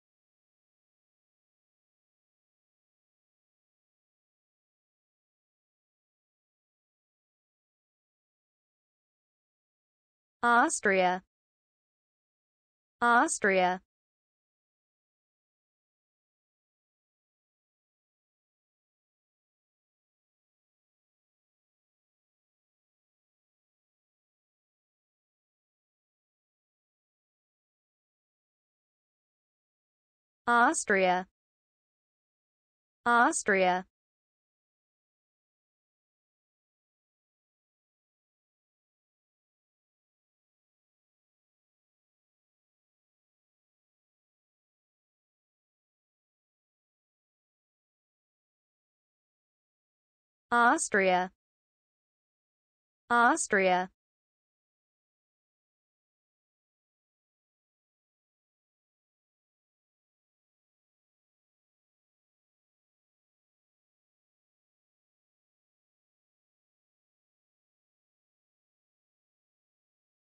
How to say Austria in English? - Pronunciation Owl
How to say Austria in English? Learn the pronunciation of Austria!